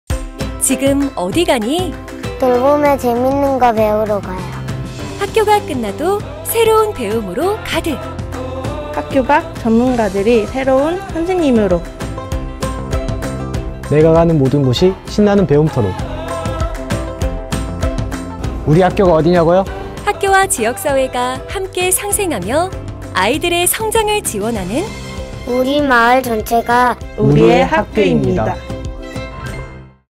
[충북교육시책 캠페인] 공동체 기반 지역교육생태계
"학교가 끝나도 새로운 배움으로 가득!"
-돌봄과 방과 후 교육을 제공하는 마을학교

"학교 밖 전문가들이 새로운 선생님으로?"
-지역연계 마을 방과 후 교육 청소년 프로젝트

"내가 가는 모든 곳이 신나는 배움터!"
-대학 등 지역사회가 함께하는 고교 교육과정

학교와 지역사회가 상생하며, 성장을 지원하는
우리 마을이 곧 우리의 학교입니다.